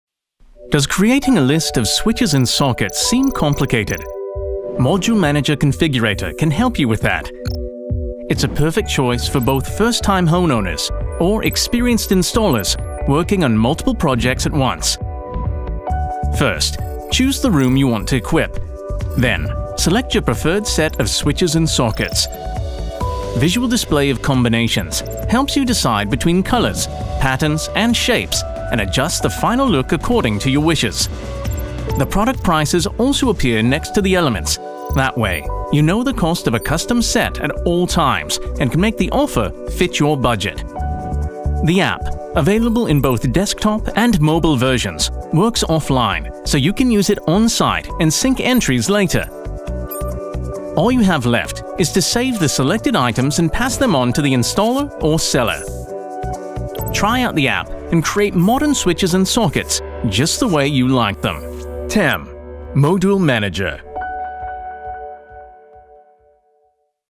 MODUL MANAGER configurator of TEM switches and sockets
MODUL MANAGER configurator/app for creating list of Modul switches and sockets.